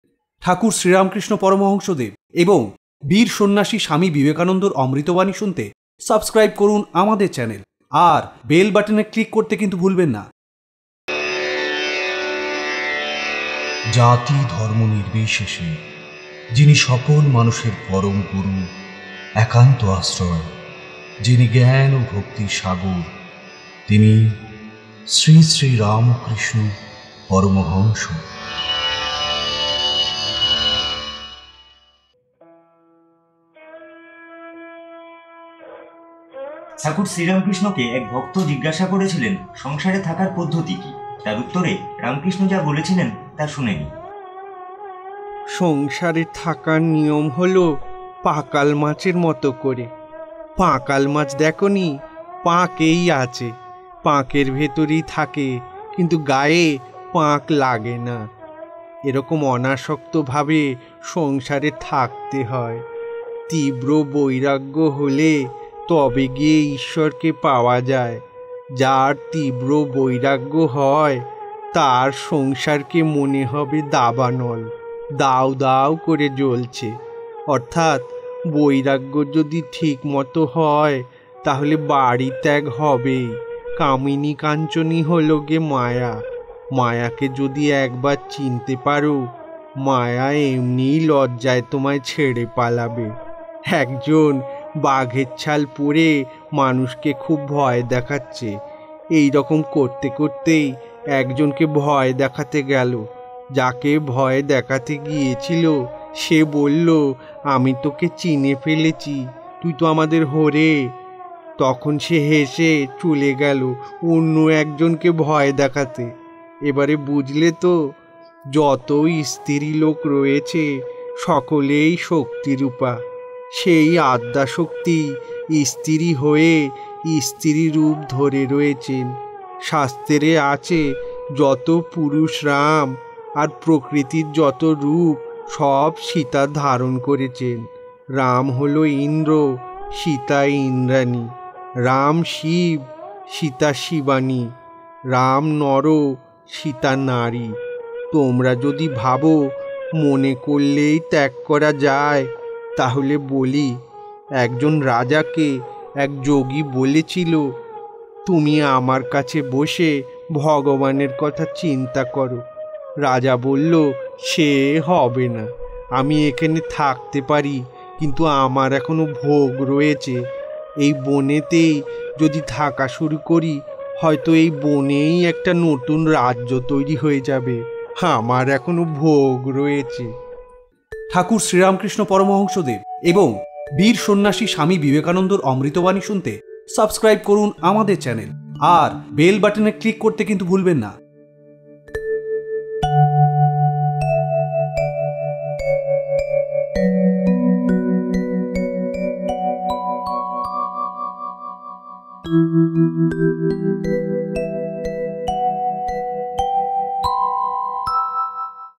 সংসারে থাকার পদ্ধতি | Kathamrita | Ramkrishna
How can we survive in family life ? Thakur Ramkrishna said that we have to live like a deep mud fish. That type of fish live in deep mud. But mud cannot touch them. One has live in family life without any attraction.